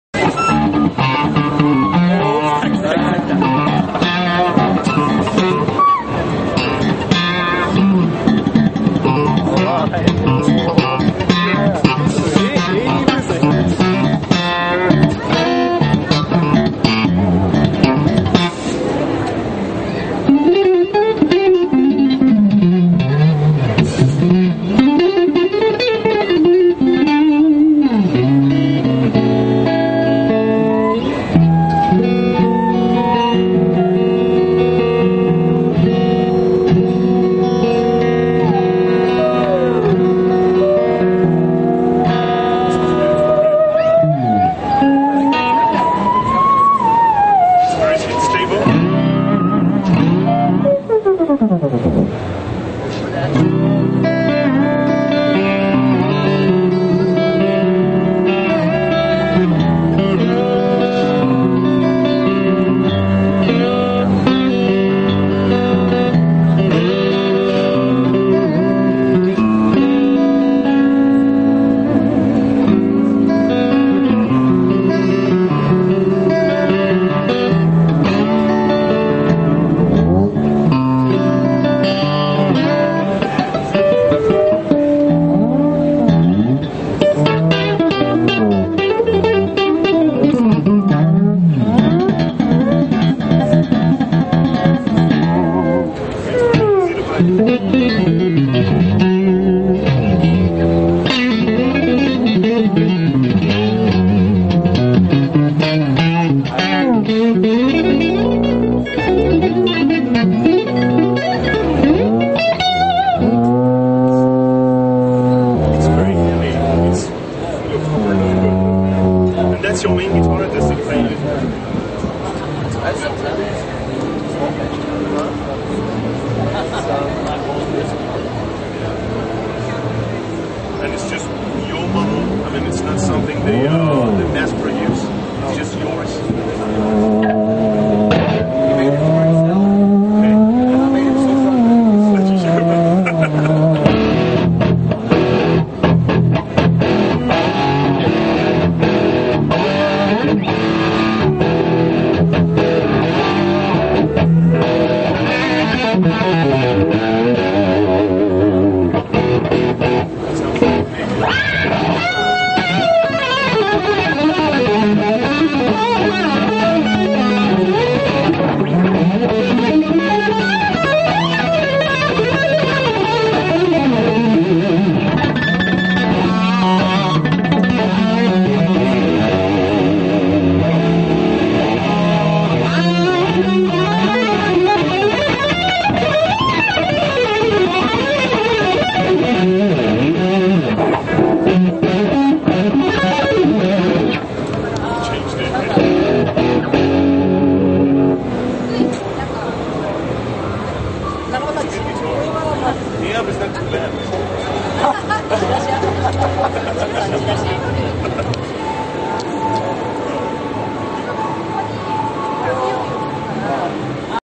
Christophe Godin in Ykohama
Kartoon Guitar Clinic for Laney at the Japan Music Fair in Yokohama